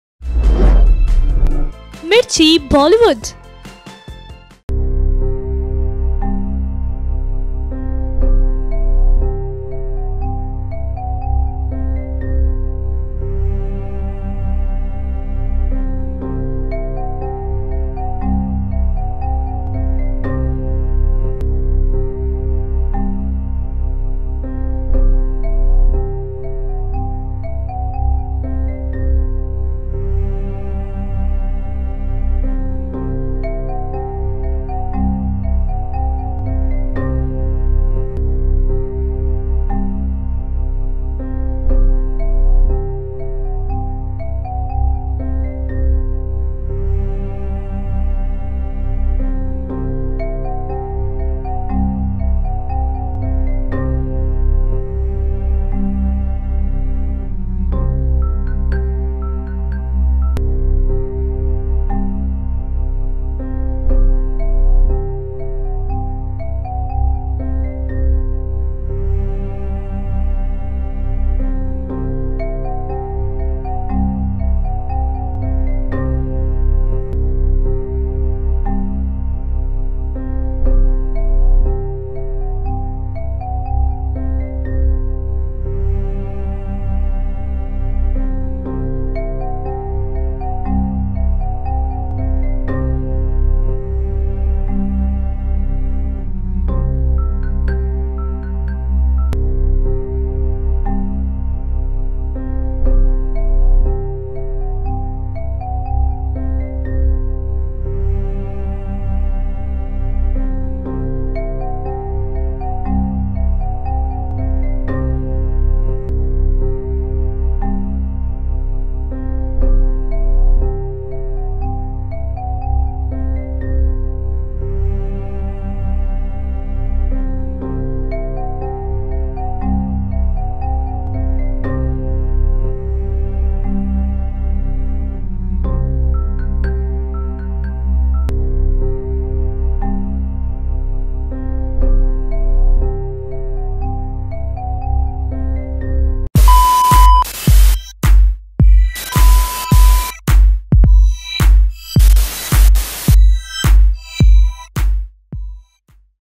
Sridevi की Death के बाद पहली बार Boney ने बयां किया अपना दर्द, चांदनी को लिखी आखिरी चिट्ठी
Look what Boney Kapoor said after the death of Sridevi